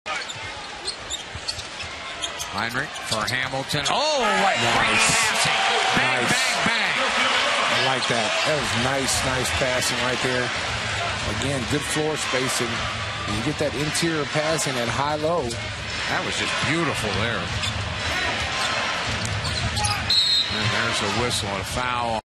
Noah's Sweet Pass | Bobcats vs Bulls | Jan 28, 2013
Joakim Noah passes the ball between the defense to Carlos Boozer for the jam.